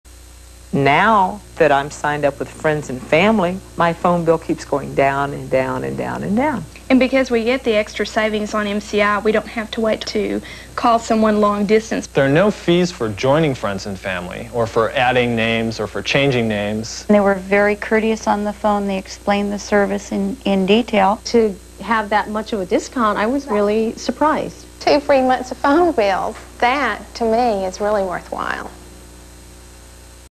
1991 MCI Long Distance "Friends and Family" TV Commercial
The commercial aired in November 3rd, 1991. It aired during a broadcast of the "America's Funniest People" on WJWA Channel 7 in Washington DC.